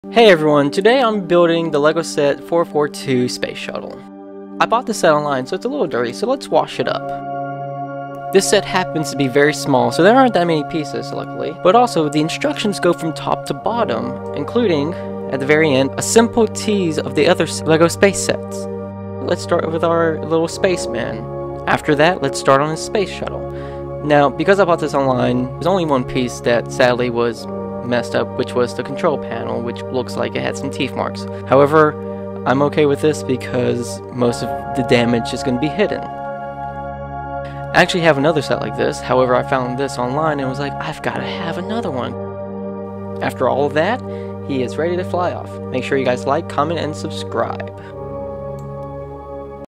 Build it: LEGO Space Shuttle (Set 442)
Short Build It video of an old LEGO Space Shuttle